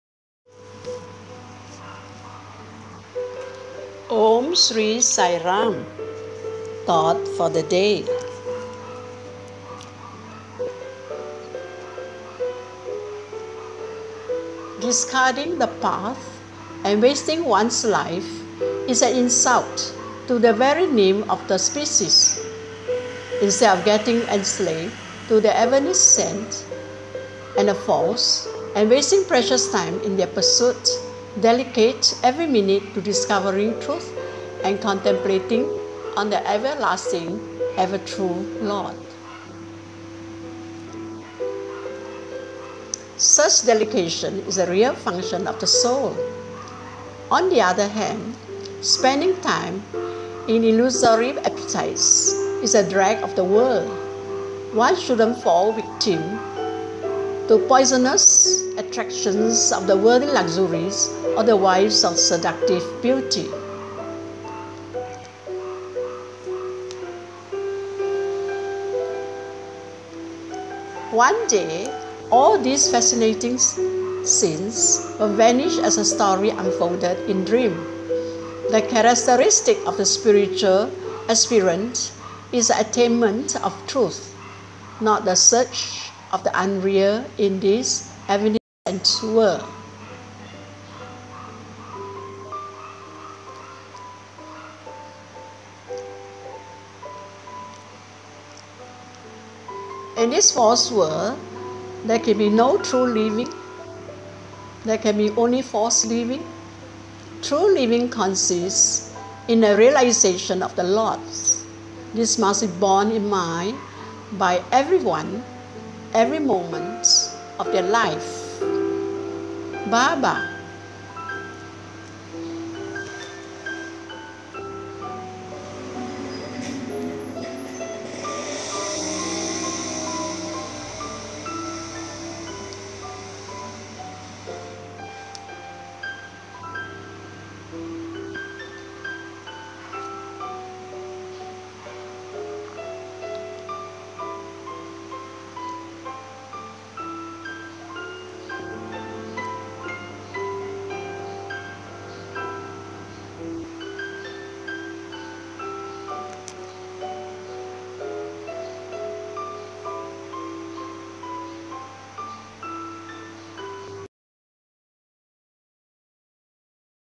Mensaje de la Pizarra de Prashanti Nilayam_30 de junio de 2015.
Activar subtitulos en el reproductor abajo a la derecha, seleccionar idioma español.
Courtesy_Radio Sai and Thought For The Day by Sai Art.

MENSAJE DEL DIA 30 de junio de 2015

Descartar el camino, y desperdiciar la propia vida, es un insulto al nombre mismo de la especie. En lugar de resultar esclavizados por lo evanescente y lo falso, malgastando el precioso tiempo en su búsqueda, dediquen cada minuto al descubrimiento de la verdad y a la contemplación en el eterno y siempre auténtico Señor. Tal dedicación es la verdadera función del alma. Por el contrario, utilizar el tiempo en ilusorios apetitos es la rémora del mundo. No debemos caer víctimas de las atracciones ponzoñosas de los lujos mundanos, ni de los ardides de la seductora belleza. Un día, todas estas escenas fascinantes se desvanecerán, como una historia desplegada en un sueño. La característica de un aspirante espiritual es el logro de la verdad, no la búsqueda de lo irreal en este mundo evanescente. En este falso mundo no puede haber auténtico vivir (sathya-achara). Sólo puede haber un falso vivir (mithya-achara). El auténtico vivir consiste en tomar conciencia del Señor. Esto debe ser tenido en cuenta por todos, en cada momento de sus vidas. (Prema Vahini, Capítulo 58)

Discarding the path and wasting one’s life is an insult to the very name of the species! Instead of getting enslaved to the evanescent and the false, and wasting precious time in their pursuit, dedicate every minute to discovering truth and contemplating on the everlasting, ever-true Lord. Such dedication is the real function of the soul. On the other hand, spending time in illusory appetites is the drag of the world. One shouldn’t fall victim to the poisonous attractions of worldly luxuries or the wiles of seductive beauty. One day, all these fascinating scenes will vanish as a story unfolded in dream! The characteristic of a spiritual aspirant is the attainment of truth, not the search of the unreal in this evanescent world. In this false world, there can be no true living (sathya-achara). There can be only false living (mithya-achara). True living consists in the realisation of the Lord. This must be borne in mind by everyone every moment of their life. (Prema Vahini Ch 58)

— BABA